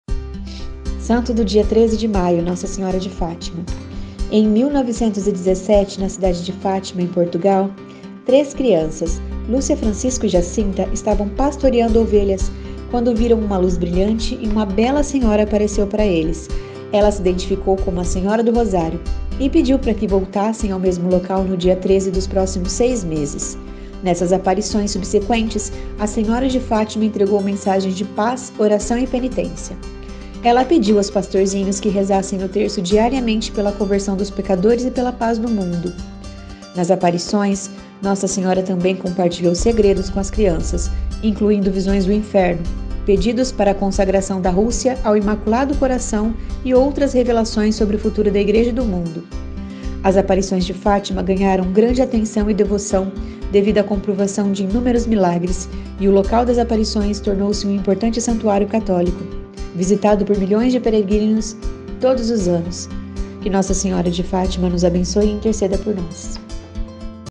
13 - 05 Nossa Senhora de Fátima
Santo do dia 13 de maio: Nossa Senhora de Fátima
Em 1917, na cidade de Fátima, Portugal, três crianças, Lúcia, Francisco e Jacinta estavam pastoreando ovelhas quando viram uma luz brilhante e uma bela senhora apareceu para eles. Ela se identificou como "a Senhora do Rosário" e pediu que voltassem ao mesmo local no dia 13 dos próximos seis meses. Nessas aparições subsequentes, a Senhora de Fátima entregou mensagens de paz, oração e penitência. Ela pediu aos pastorzinhos que rezassem o terço diariamente pela conversão dos pecadores e pela paz no mundo. Nas aparições, Nossa Senhora também compartilhou segredos com as crianças, incluindo visões do inferno, pedidos para a consagração da Rússia ao seu Imaculado Coração e outras revelações sobre o futuro da Igreja e do mundo. As aparições de Fátima ganharam grande atenção e devoção devido a comprovação de inúmeros milagres, e o local das aparições tornou-se um importante santuário católico, visitado por milhões de peregrinos todos os anos. Que Nossa Senhora de Fátima nos abençoe e interceda por nós.